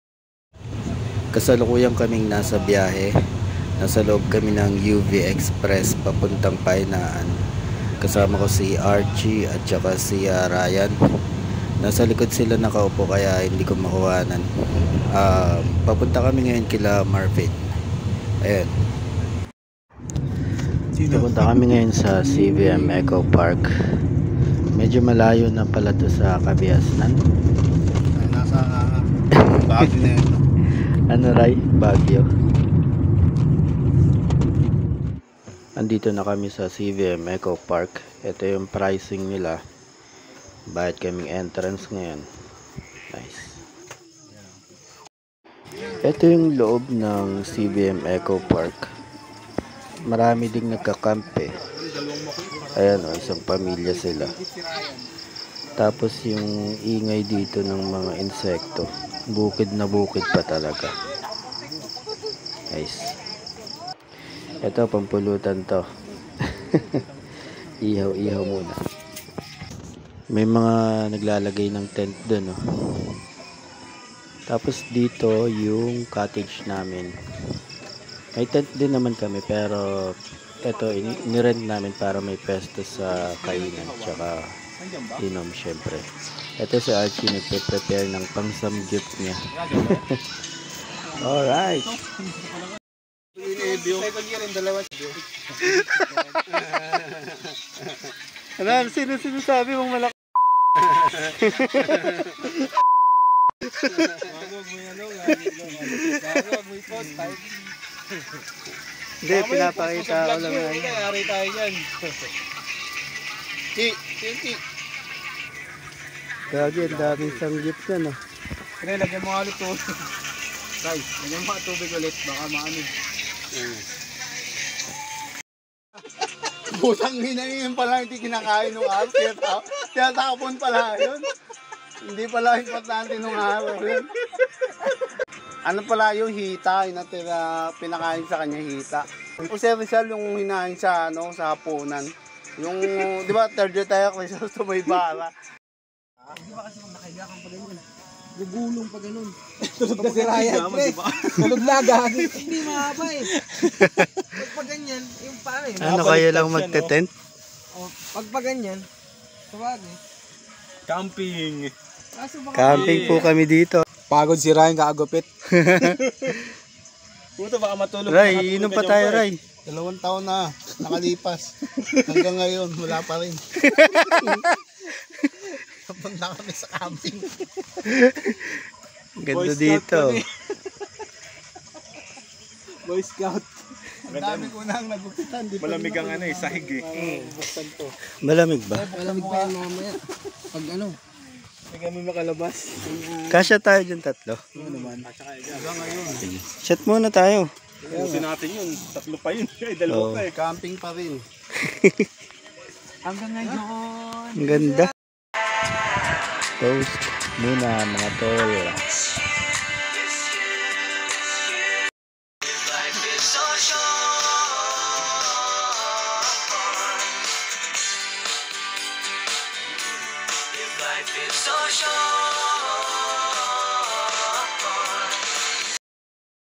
CAMPING
Napaka-ikling vlog tungkol sa biglaang camping kasama ang mga High-School friends sa CVM ECO Park. Baras, Rizal.

Footage taken: July, 18, 2021

In attendance:

Marvin Amul
Archie Gaoiran
Ryan Rodriguez
Angelo De Sierra

[Extra]
Wilbert Ordinario